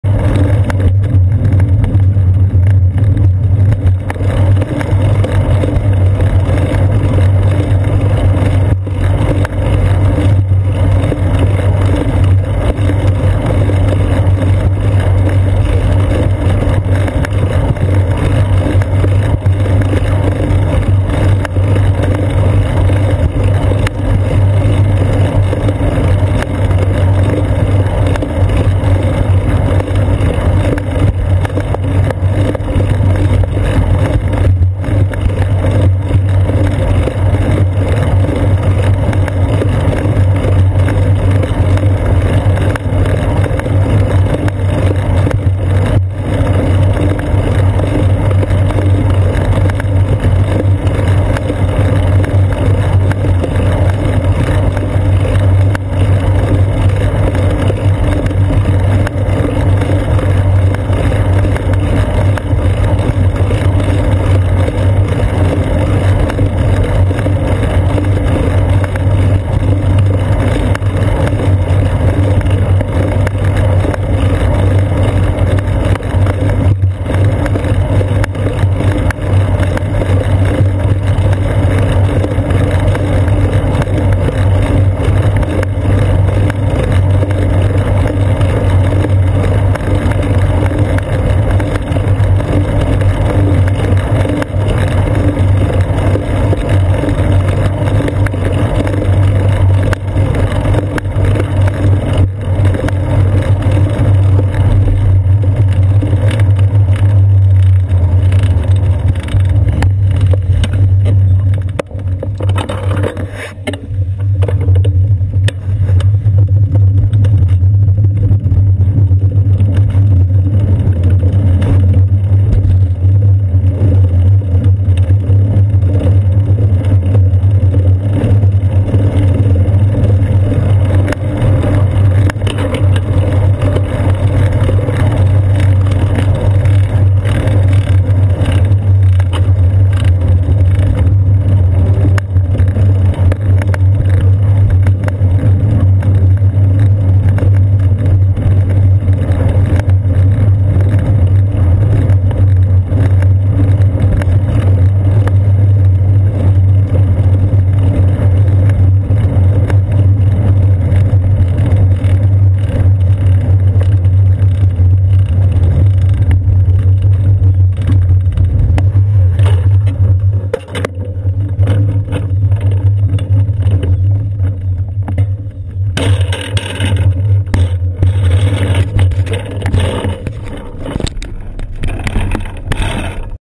Florida Bike commute Thursday morning - Quiet ride - rear camera, part 2/2
There were not many people out an about (perhaps due to the local schools being on spring break) making for quiet biking through the streets of Safety Harbor and Clearwater, Florida on the way to work on the morning of April 02, 2015.